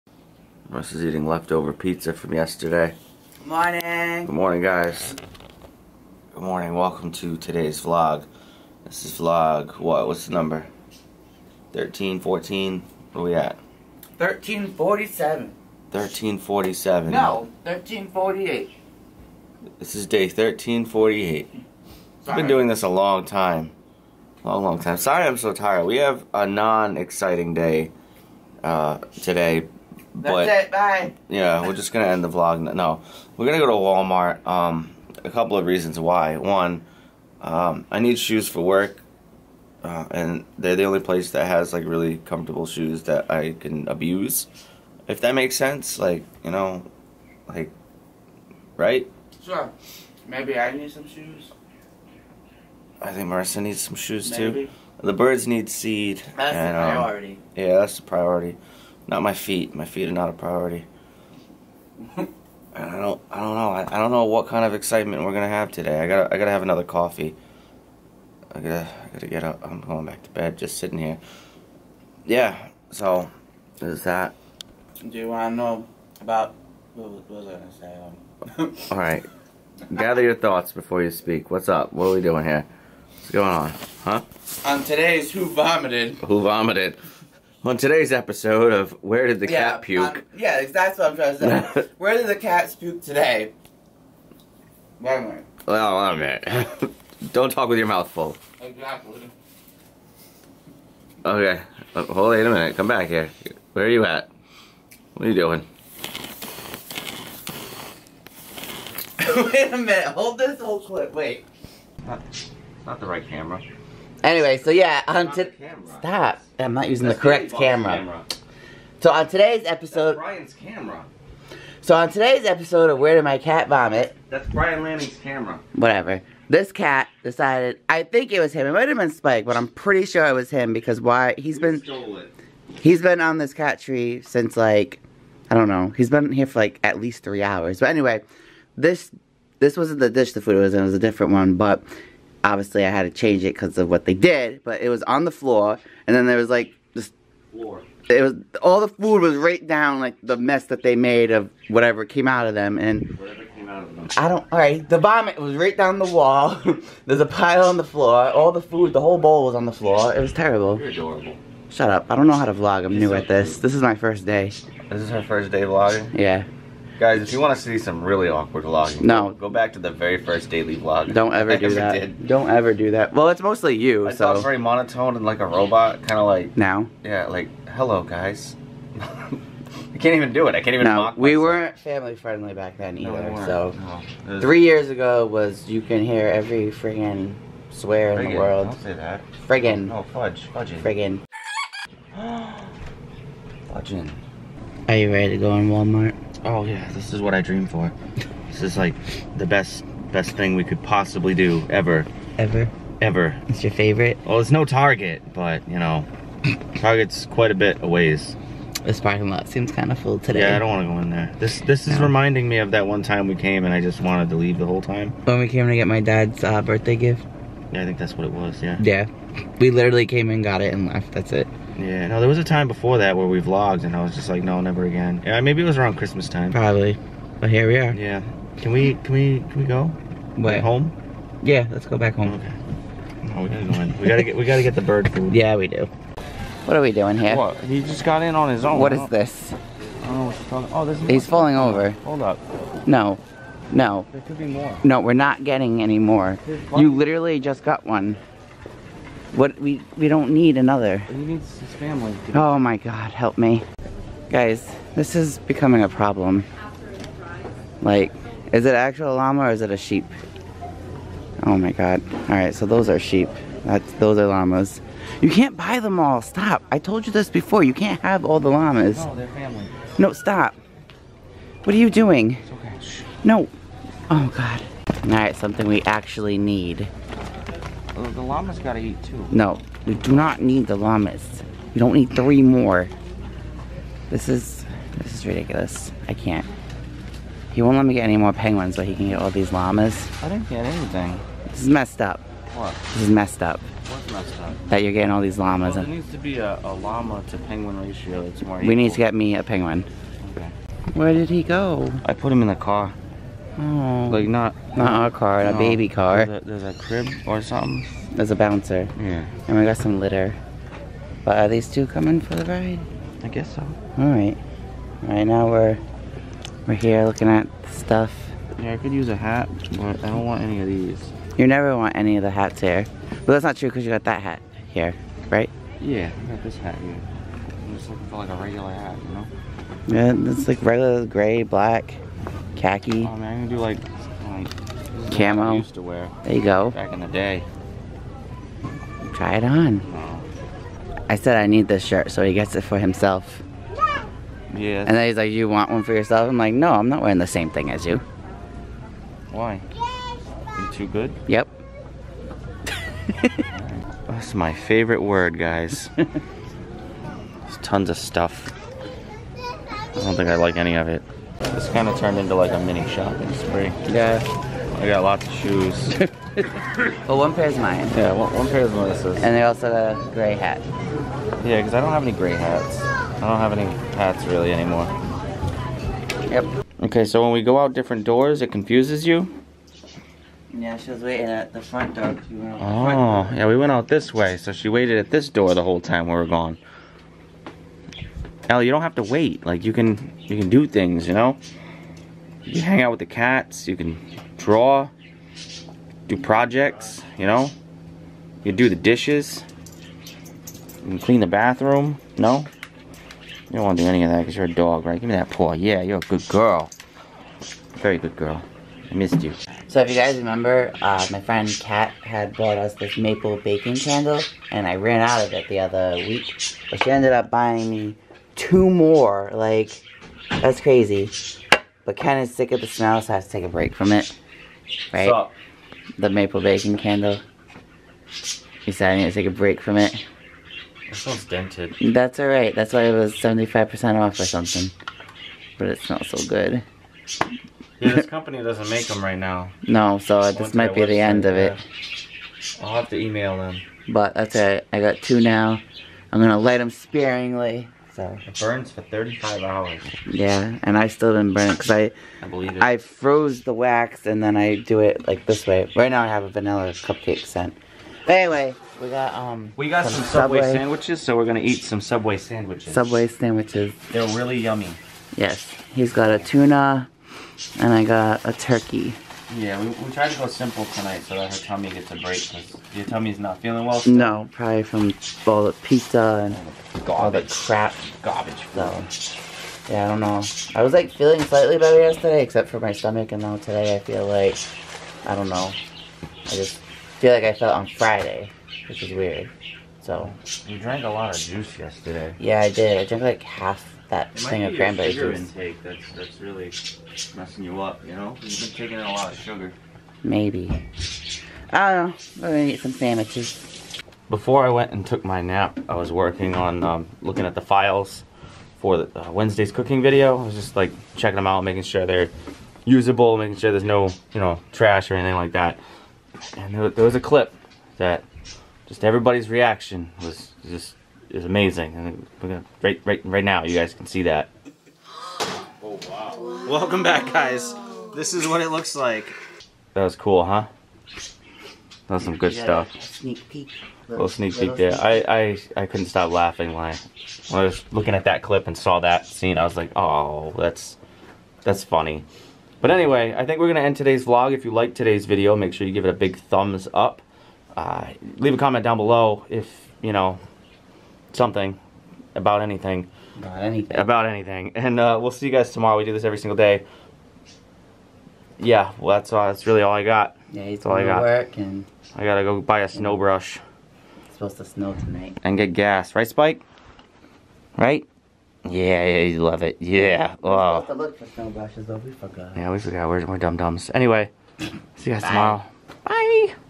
You Can't Have All The Llamas | Vlog 1348
02.17.19 | Day 1348

We go to Walmart for a few a things, Ken can't resist the llamas, & we show you a sneak peek of the upcoming cooking video.

(Warning: There may be explicit content!)

Ken 👻: tkoprovidence
Marissa 👻: miffy.penguin

PODCASTS:
(BAD ADVICE)